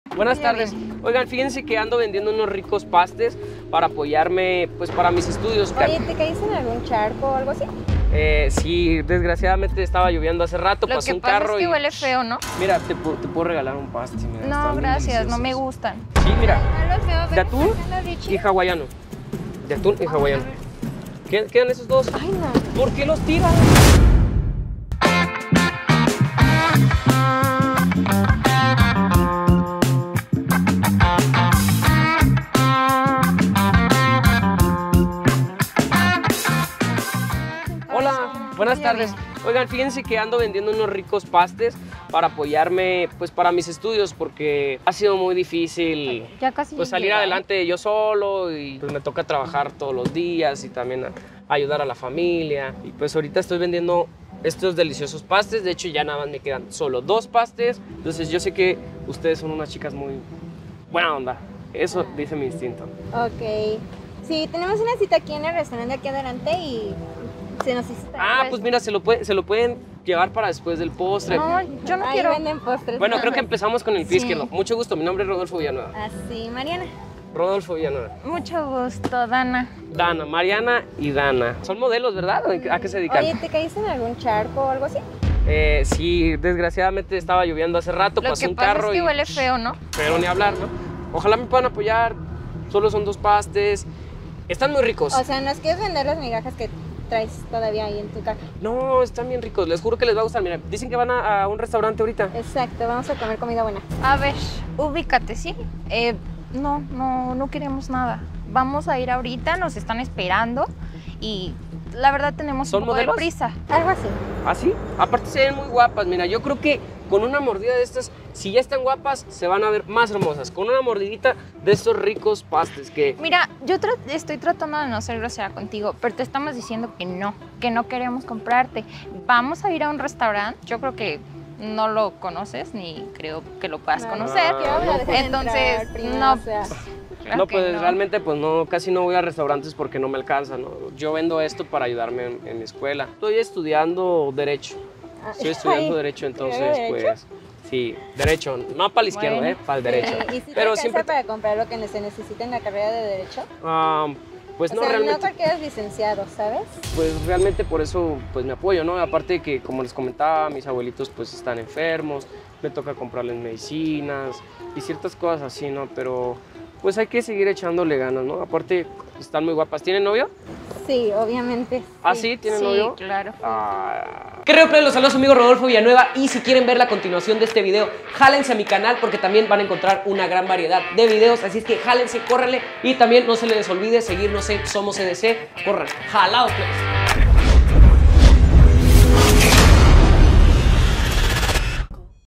INTERESADAS me HUMILLAN por VENDER EMPANADAS y SE SORPRENDEN AL VER MI AUTO DEPORTIVO
​ @Rodolfo Villanueva
Dos pájaros de un tiro, aguas plebes las interesadas no paran 😎

🔥🔥🔥 ¡JALADOS PLEBES! 🔥🔥🔥